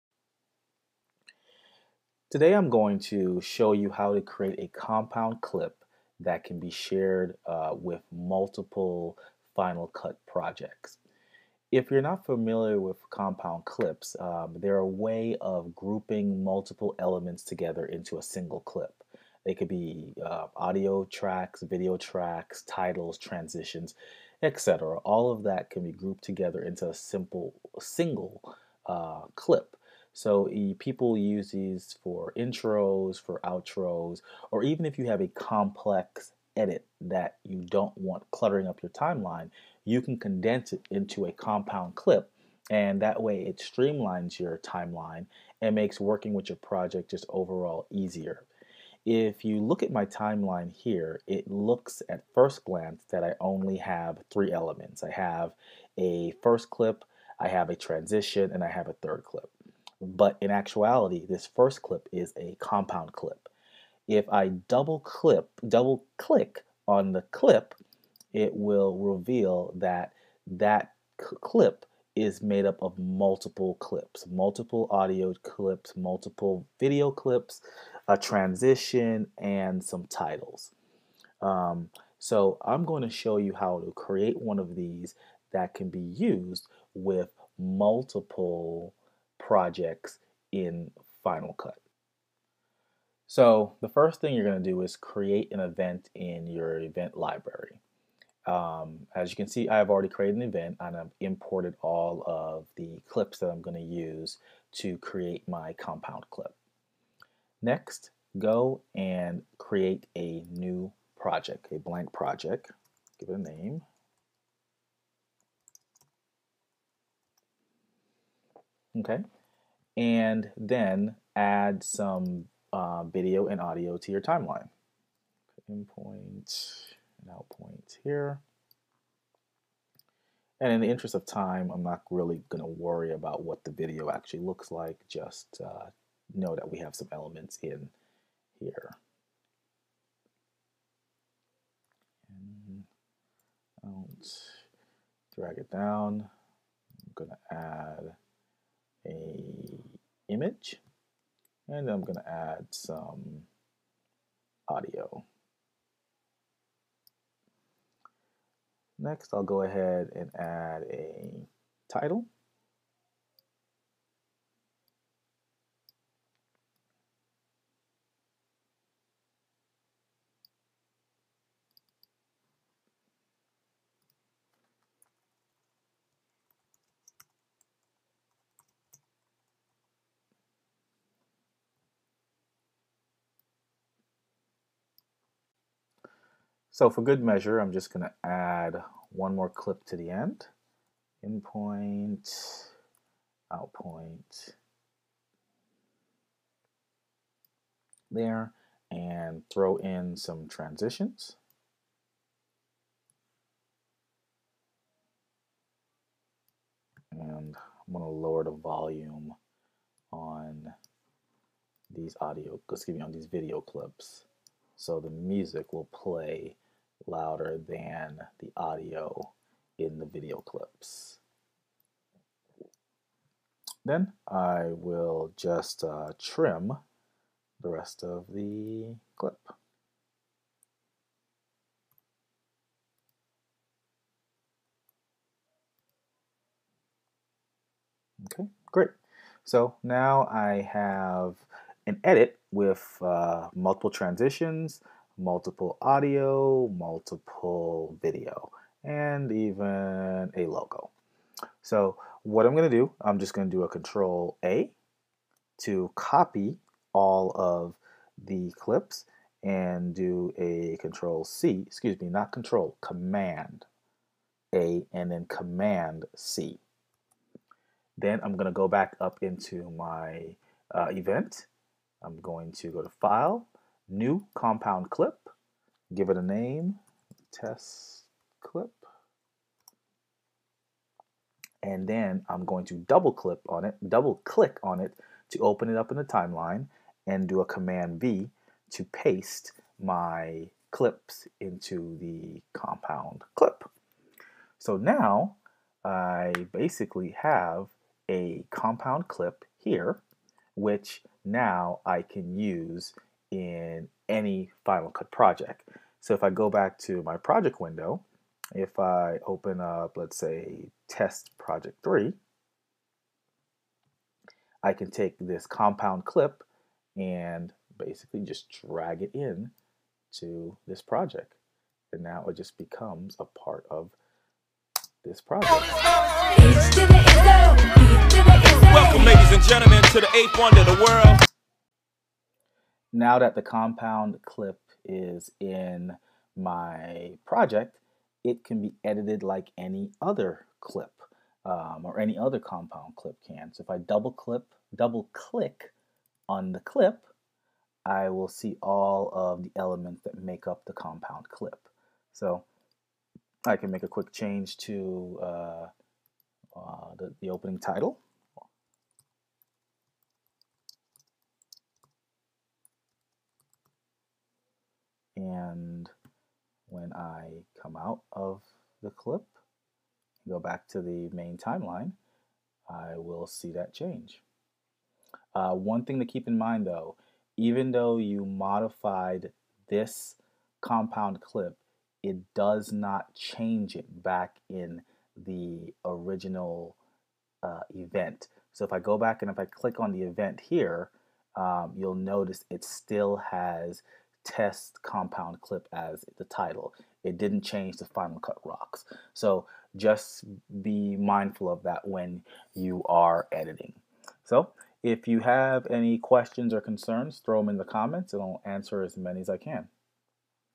Final Cut Pro X - Share Compound Clips Between Projects
This tutorial guides you through creating compound clips that can be shared between projects in  Final Cut Pro X.